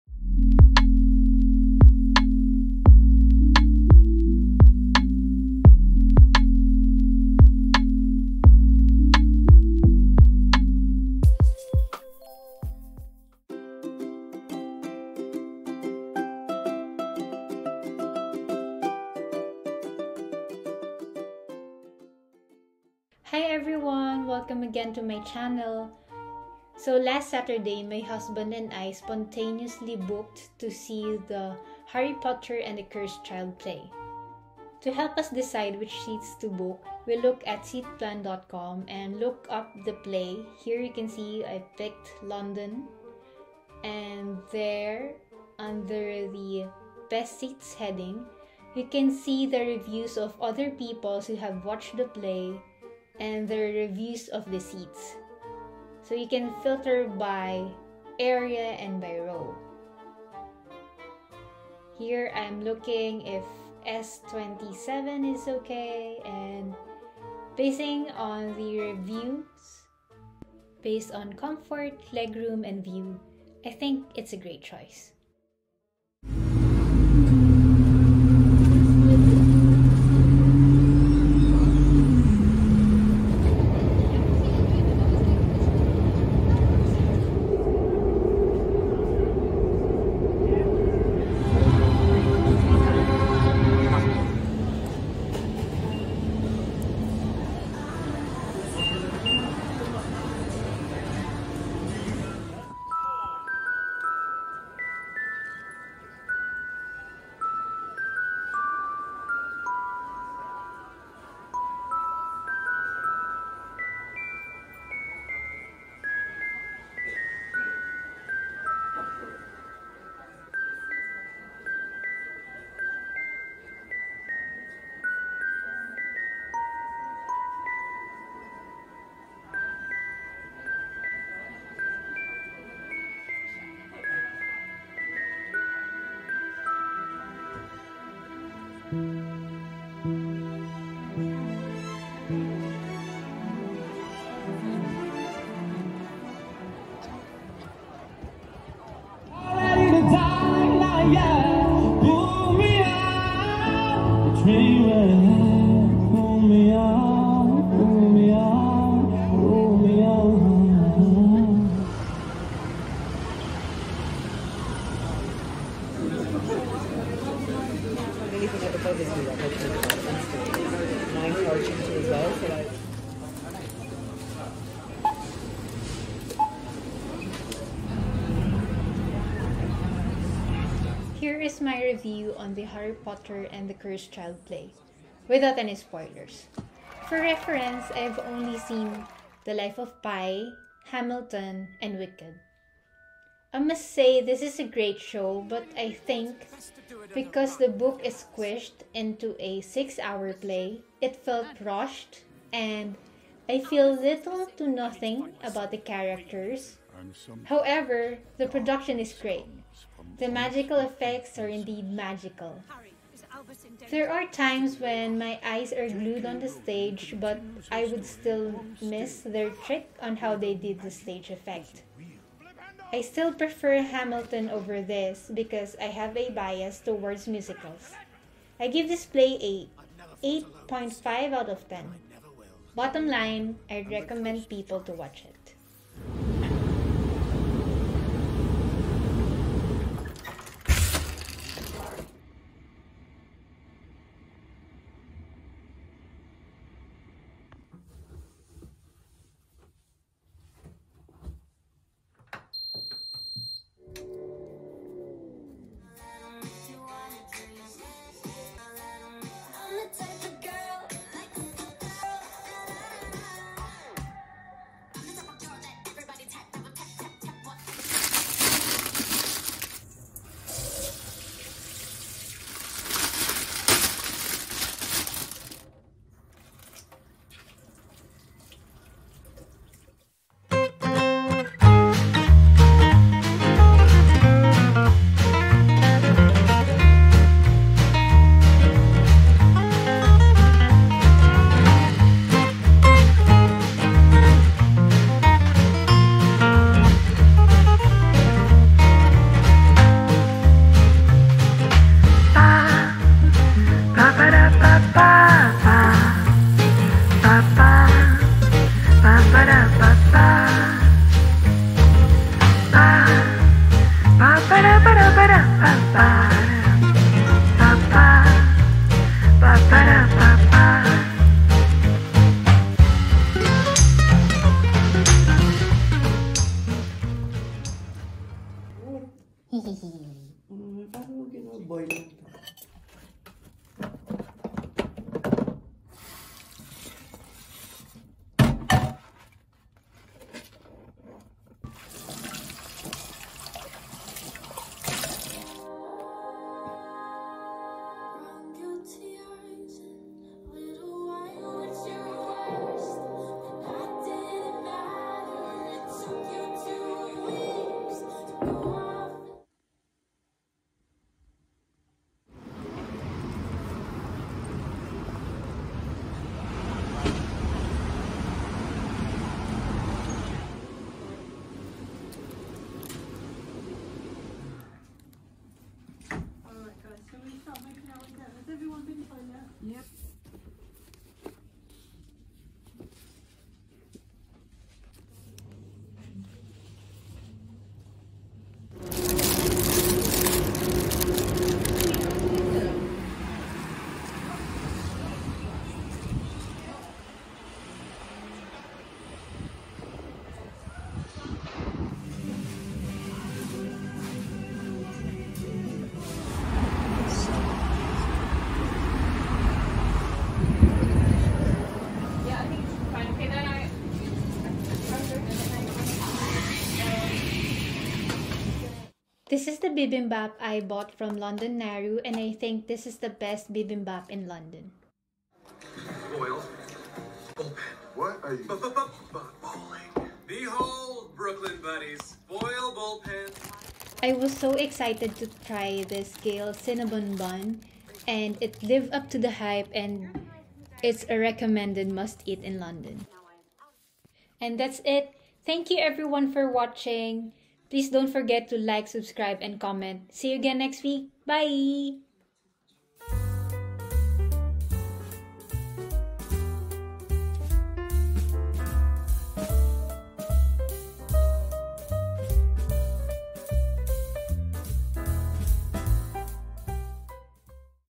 london housewife series | episode two
Hi there! Here's a peek at my week as a housewife based in London PLUS a review on the Harry Potter and the Cursed Child play!